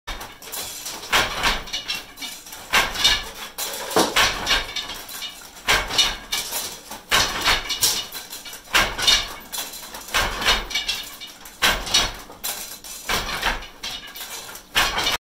Weaving Plain Weave Towels
Plain weave is the simplest weave structure, which means it weaves very quickly.